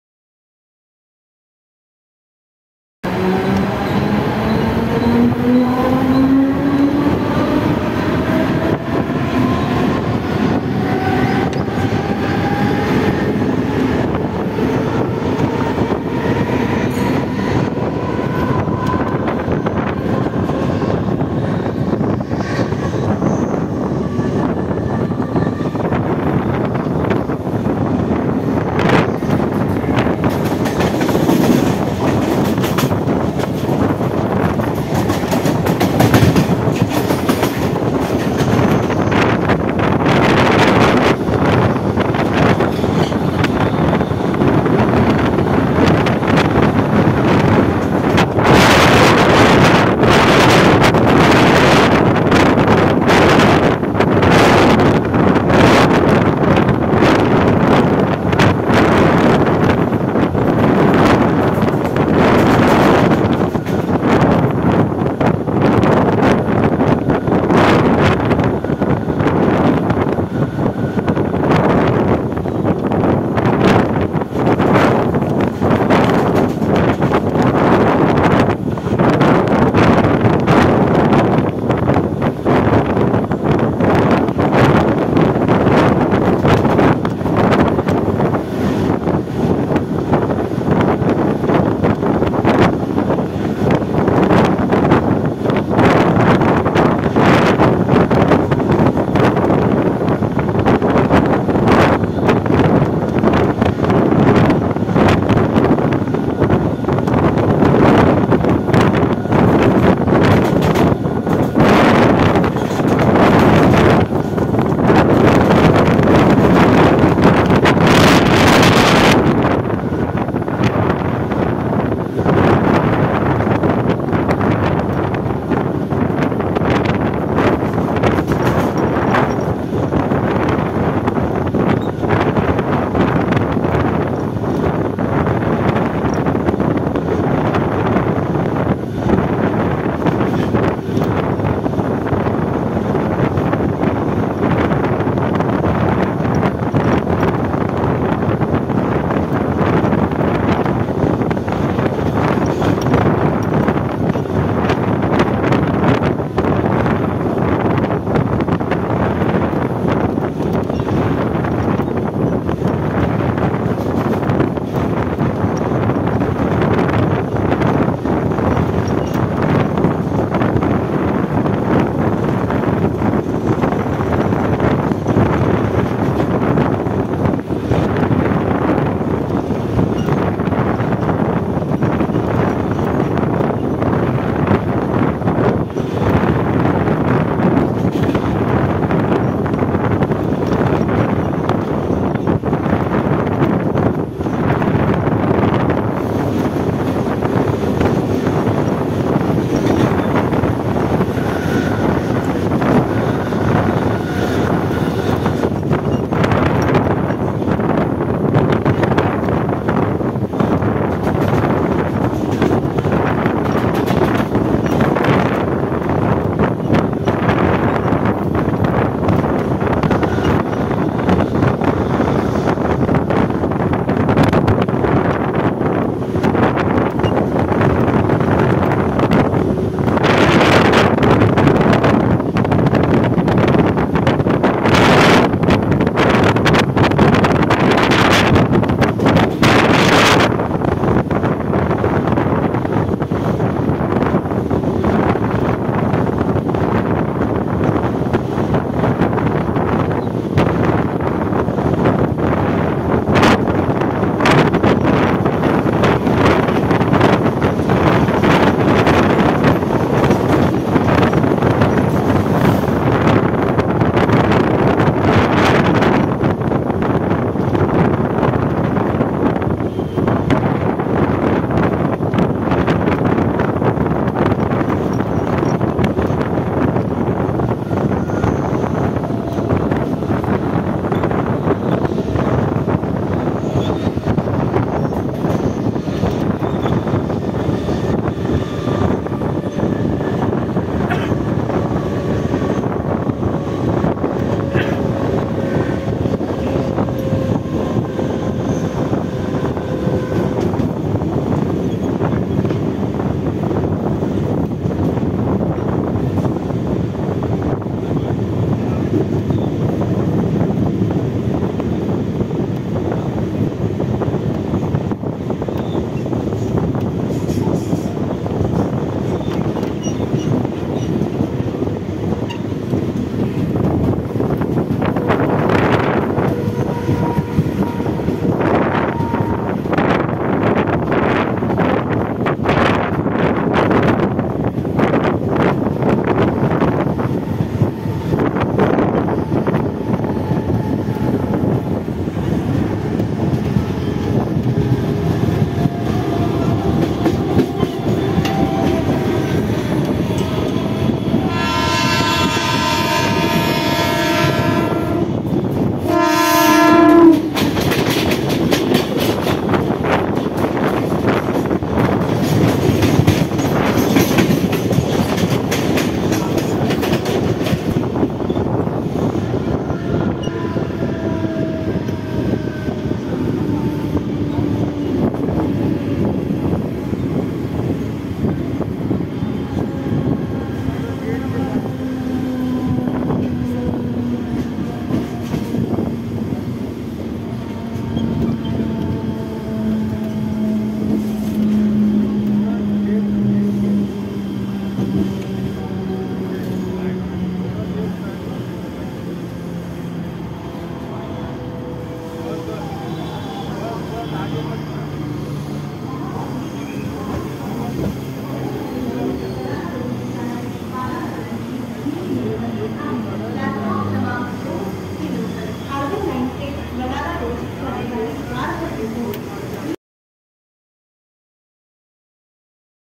Mumbai EMU Train Acceleration Speed journey between Vashi to Mankhard Harbour Line
Hourbour Line Mumbai EMU Train